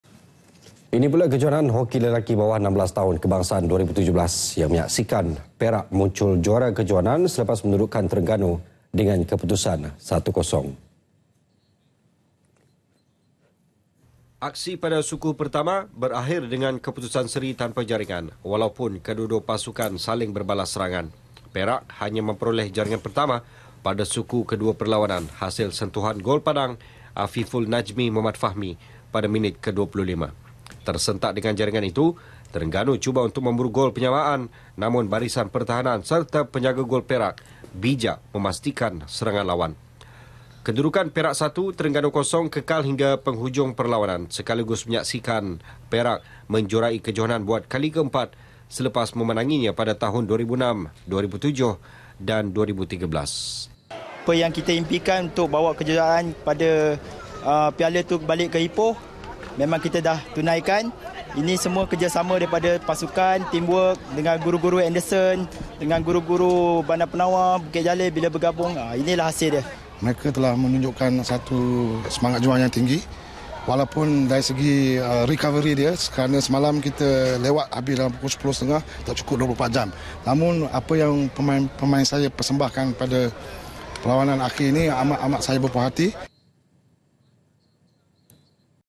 KEJOHANAN HOKI LELAKI BAWAH 16 TAHUN - PERAK 1 , TERENGGANU 0 [27 MAC 2017]
Kejohanan hoki lelaki bawah 16 tahun kebangsaan 2017 menyaksikan Perak muncul juara kejohanan selepas menundukkan Terengganu dengan keputusan 1-0.